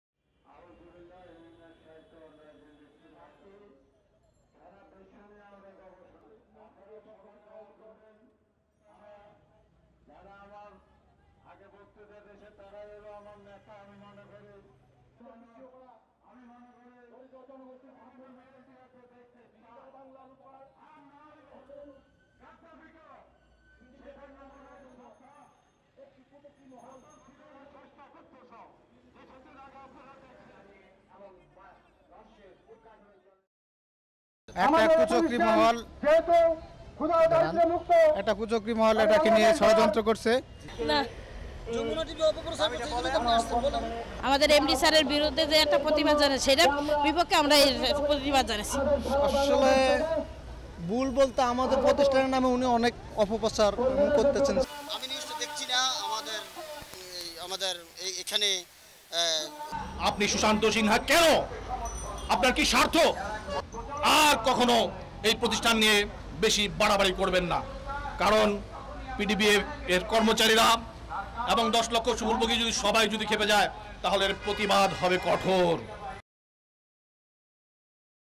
সাংবাদিককে হুমকী | Threat to Journalist | Sushanta sinha
Jamuna Television on aired some investigation story regarding the corruption of PDBF MD. After that, Amirul Hasan, pocket leader of Running MD mahbubur rahman called a human chain in front of press club and threaten to me to not cover any news. They told that If I produce any news in future i will be punish.